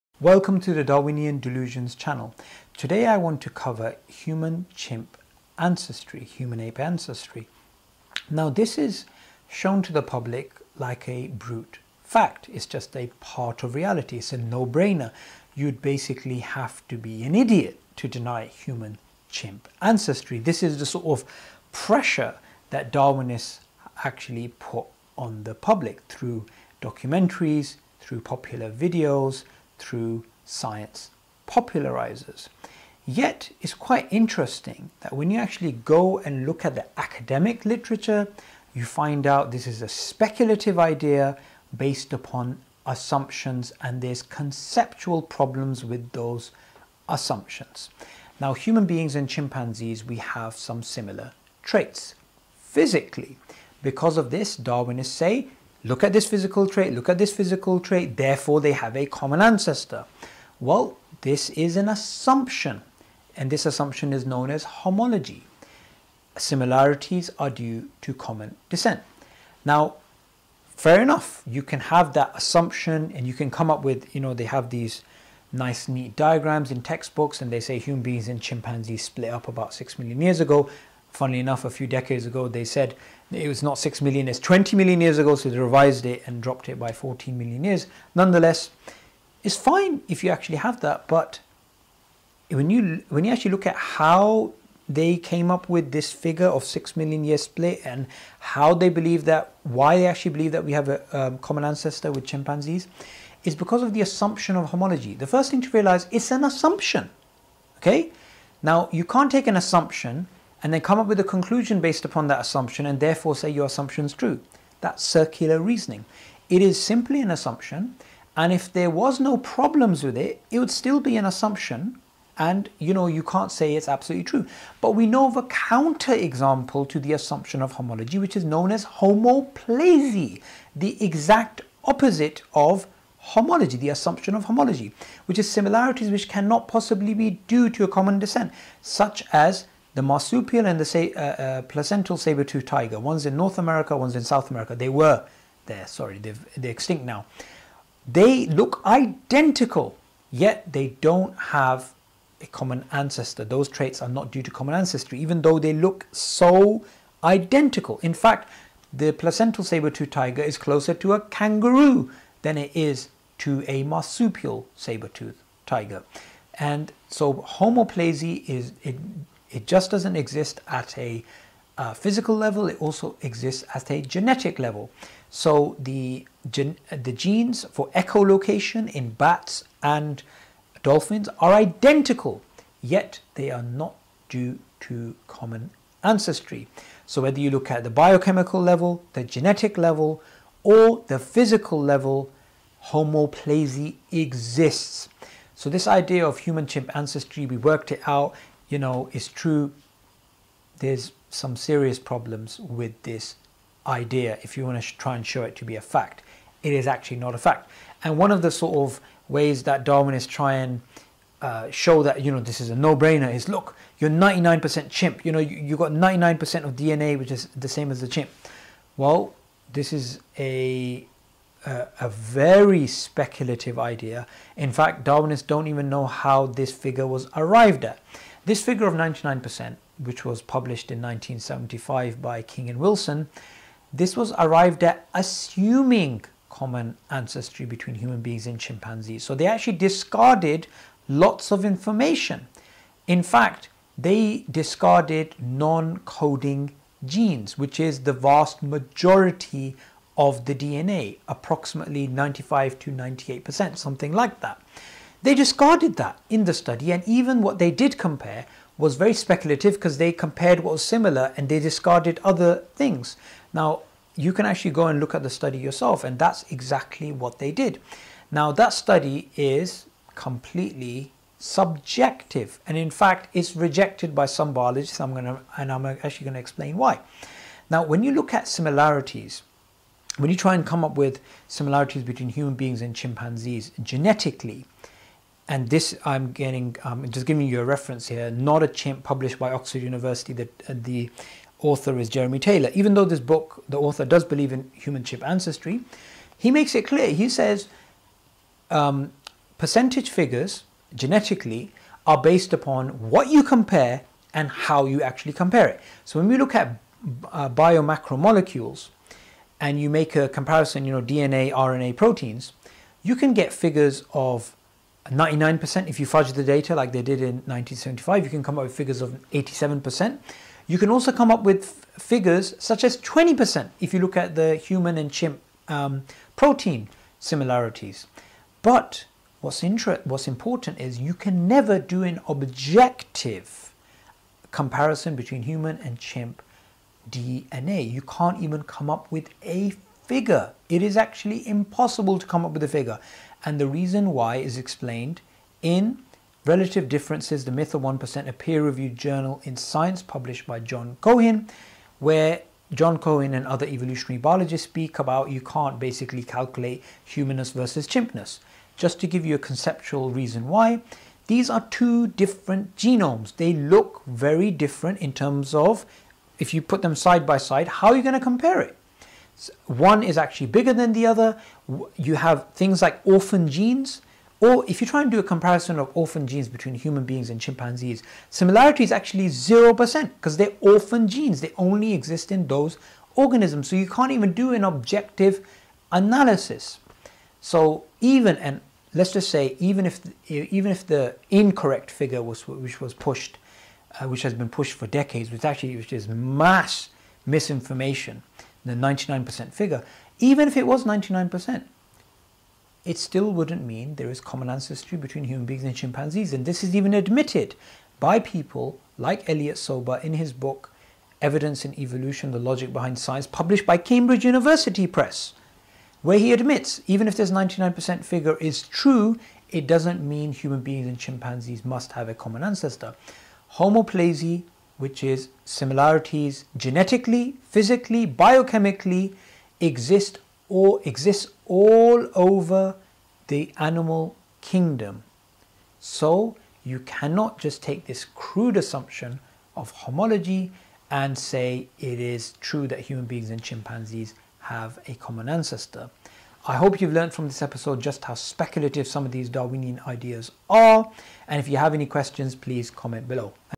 Is it true humans and Apes are related? || Darwinian theory and Reality
Brother Subbor Ahmed explain Darwinian theory it's reality and answer the question that wether humans are related to Apes.

Amazing Video watch till the end.